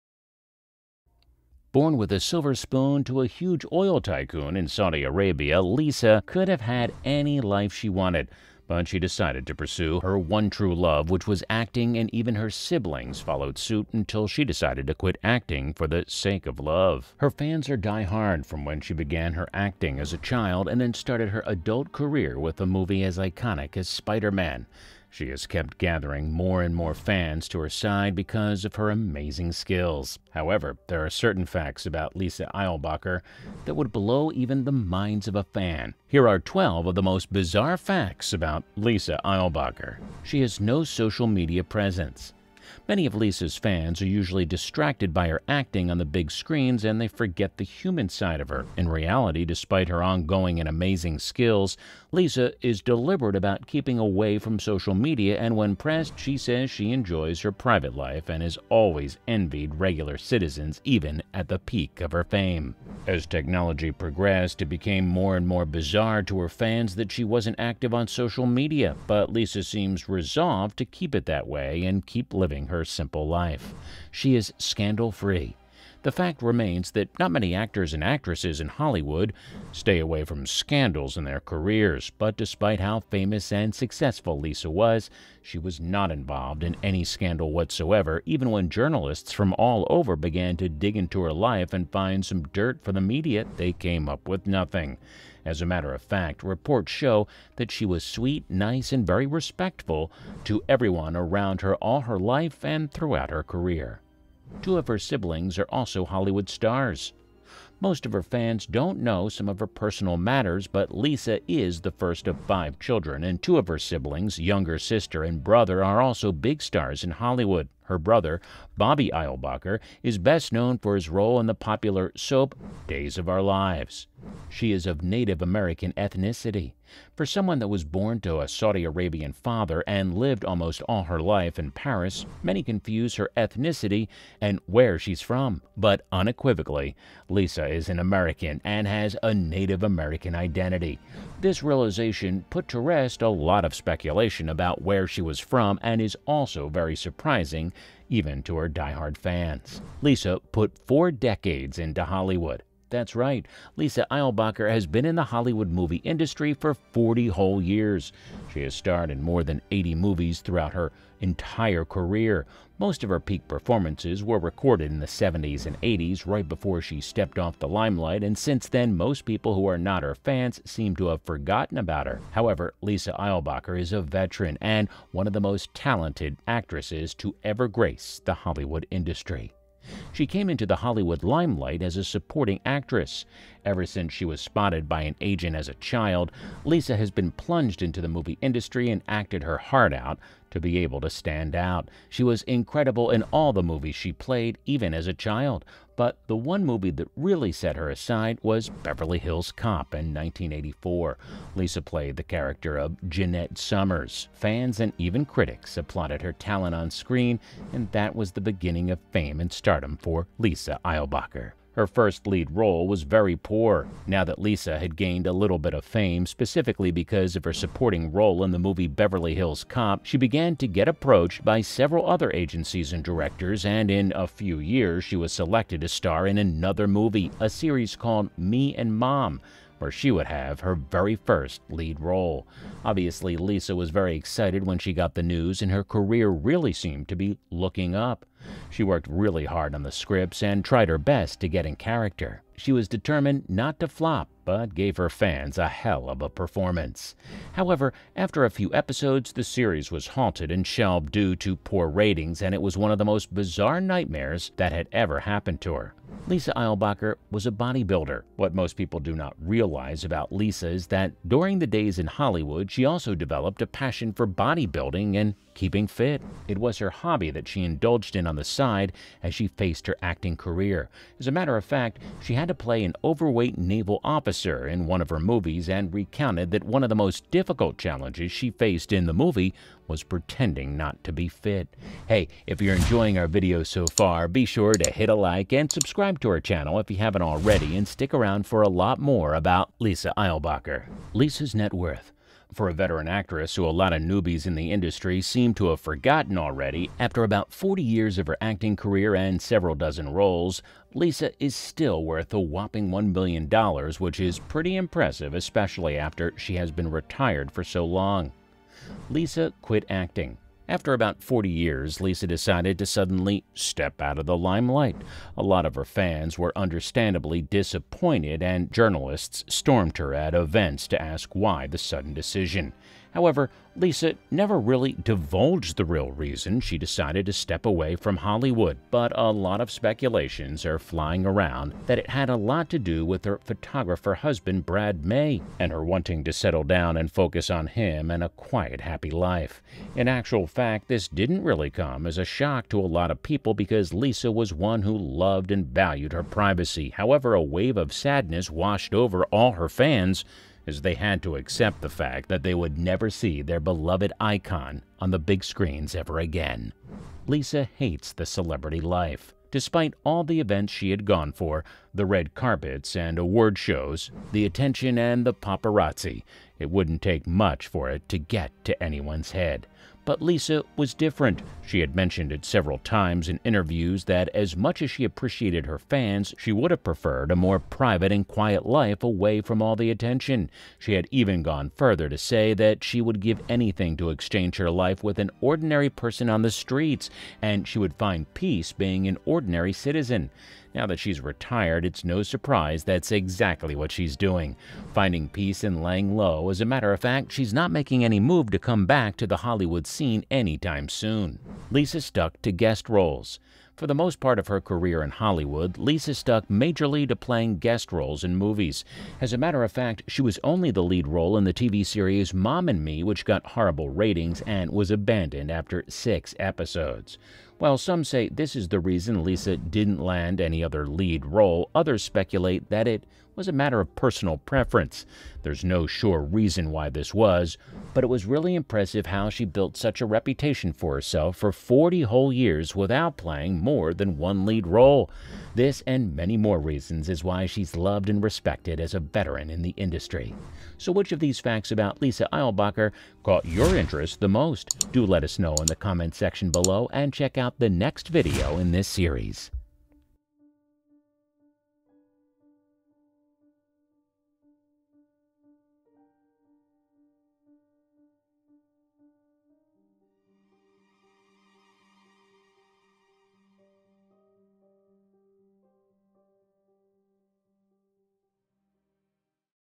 12 Bizarre Lisa Eilbacher Facts You Need to Know
Born with a silver spoon to a huge oil tycoon in Saudi Arabia, Lisa could have had any
life she wanted, but she decided to pursue her one true love which was acting and even
her siblings followed suit until she decided to quit acting for the sake of love.

00:00 - Intro
00:41 - No media presence
02:08 - She's a native American
02:32 - Has a 4 decades career in Hollywood
03:02 - Started as a supporting actress
03:33 - First performance was pretty poor
04:19 - She was a bodybuilder
04:53 - Her net worth
05:11 - Quit acting
06:00 - Hates celebrity life
06:45 - Stuck to guest roles

Many of Lisa’s fans are usually distracted by her acting on the big screens that they
forget the human side of her. In reality, despite her outgoing and amazing skills, Lisa
makes is deliberate about keeping away from social media and when pressed, she says
 she enjoys her private life and has always envied regular citizens even at the peak of
her fame.
As technology progressed, it became more and more bizarre to her fans that she wasn’t
 active on social media, but Lisa seems resolved to keep it that way and keep living her
 simple life.

12 Bizarre Lisa Eilbacher Facts You Need to Know